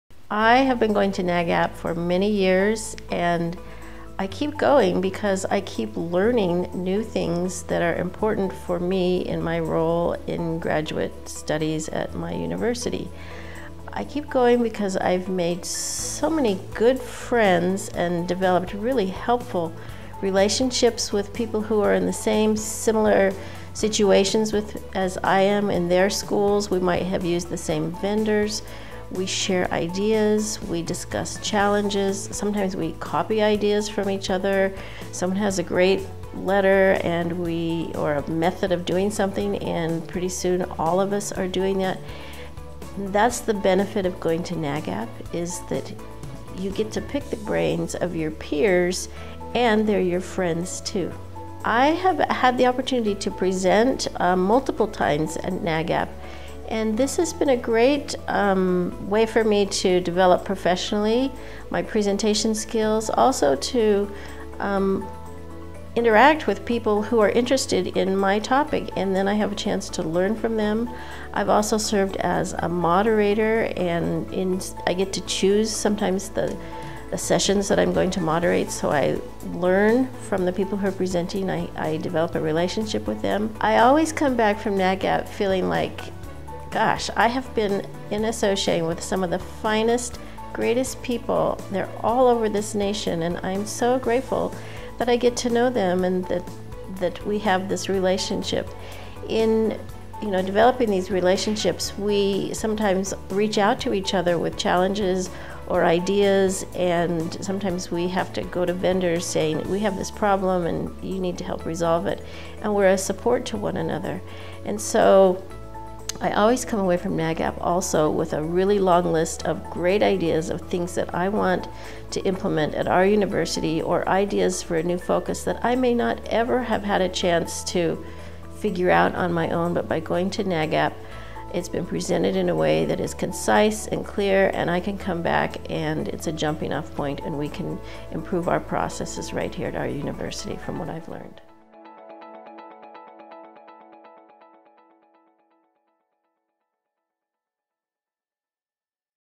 WhyINAGAP Janice Robinson BYU
Janice Robinson from BYU shares her story of WhyINAGAP